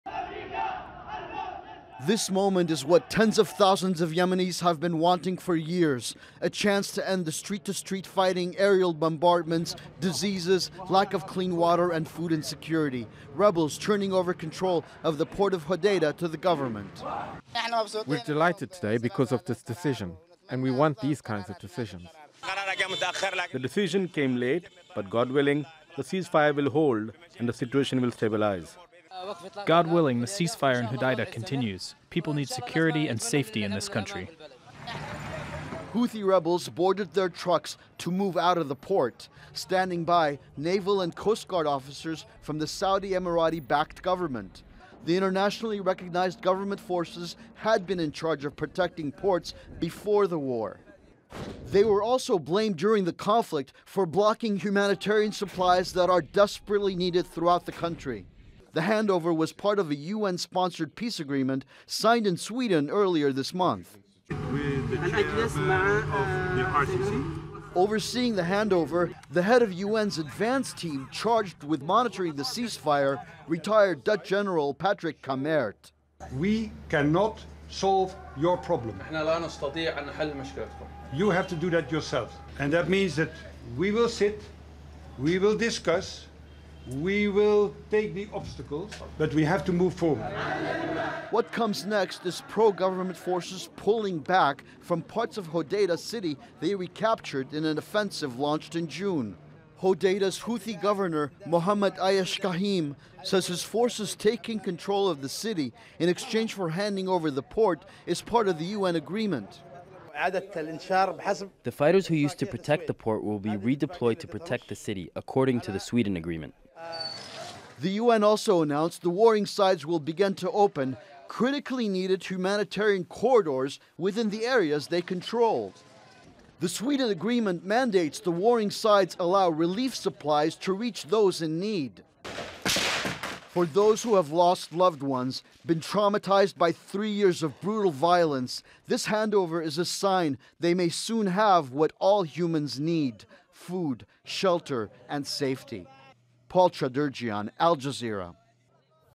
🇾🇪Yemen truce: Houthi rebels give control of Hodeidah port to navy | Al Jazeera English
Yemen's Houthi rebels have handed over control of the vital port of Hodeidah to government forces.
The transfer of the port of Hodeidah and two others is part of a peace deal reached in Sweden earlier this month, aimed at ending almost four years of war which has caused a humanitarian disaster.
Houthi rebels and Saudi-backed forces have also agreed to open up aid corridors.

Al Jazeera's Paul Chaderjian reports.